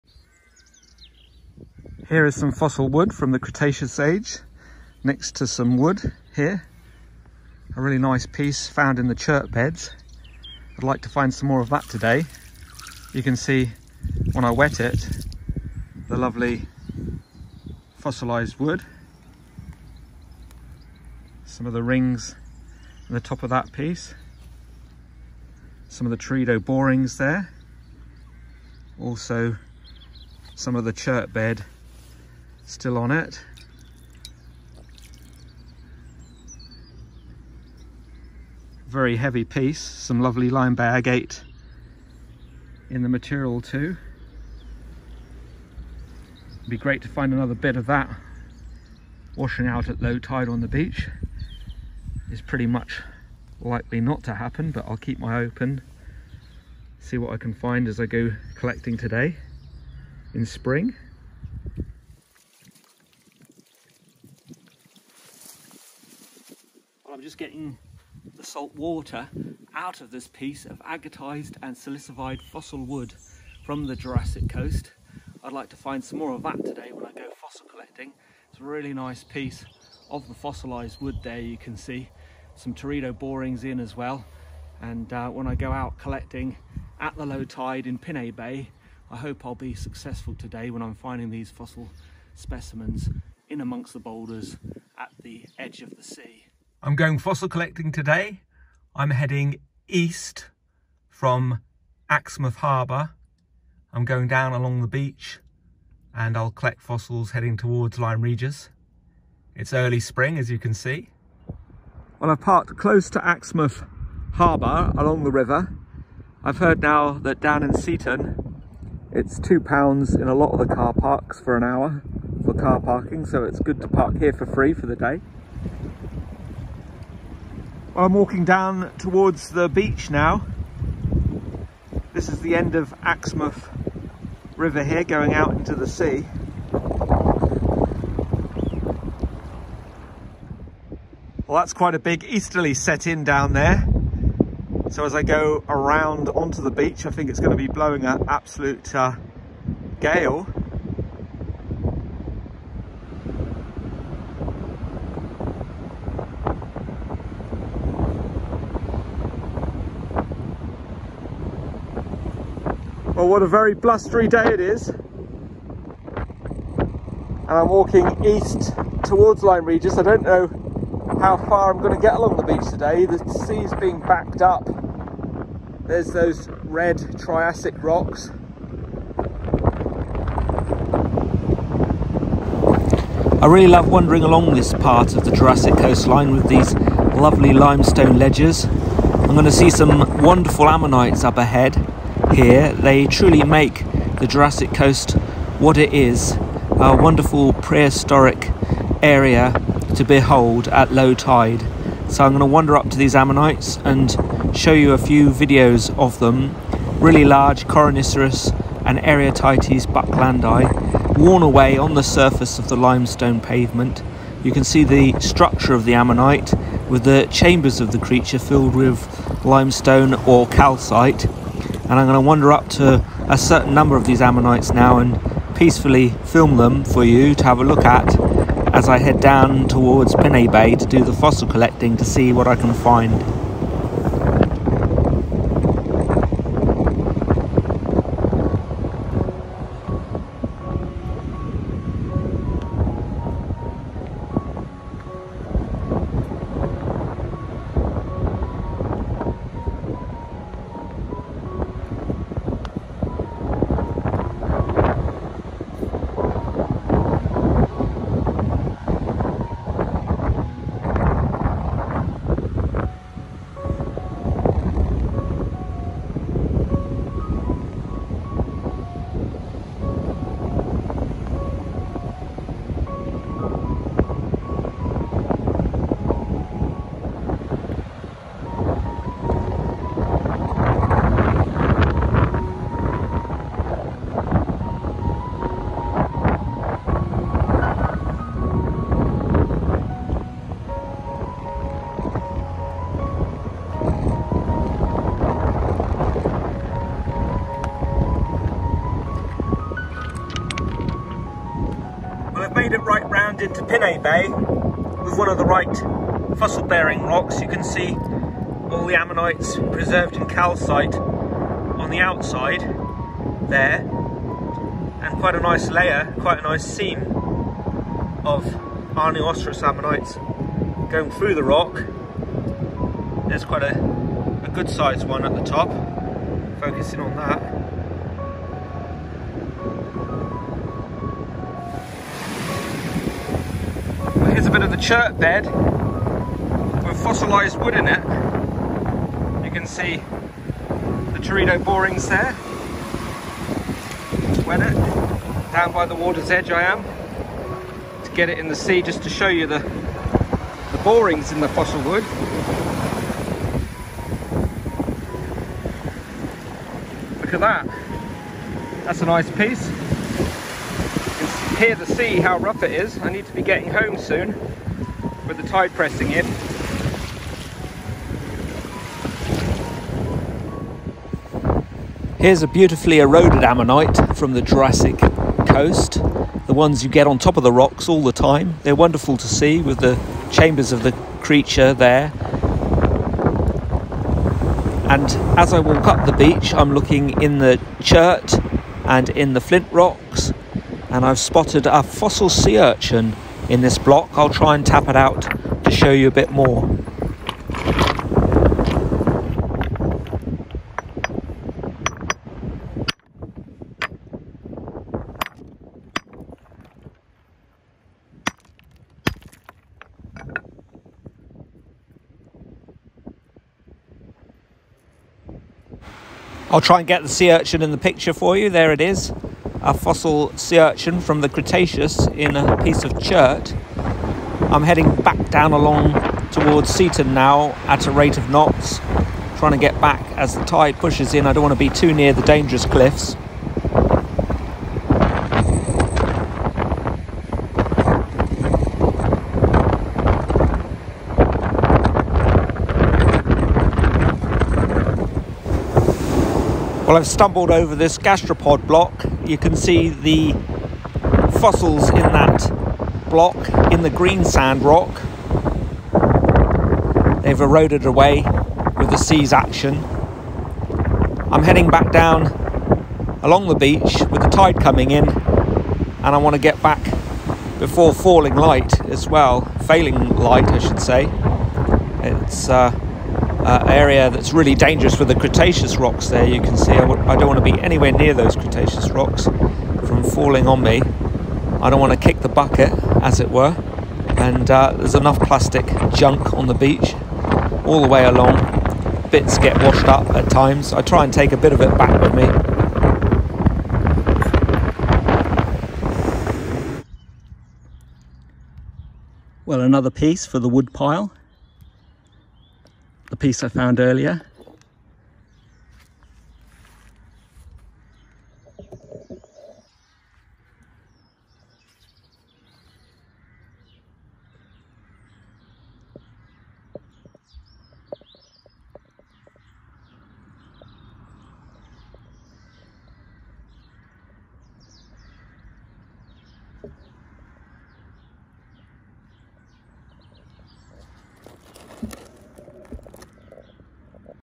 Wood Turned Into Stone & Awesome Ammonites.
Here is some fossil wood from the Cretaceous age, i found in the chert beds. I am going fossil hunting today along the Jurassic Coast- at low tide. I recommend wearing safety glasses and sturdy rubber gloves when hammering the right rocks. Remember the cliffs are dangerous and liable to fall suddenly and without warning. Keep away from the dangerous cliffs the best fossils are washed out onto the beaches at low tide by the sea's actions. Look out for the slippery seaweed on the ledges and rocks. I offer fossil walks along the Jurassic Coast on the following website, so feel free to take a look at the website I made, with our online fossil walks There is a fossil collecting code of conduct in place along the Jurassic Coast.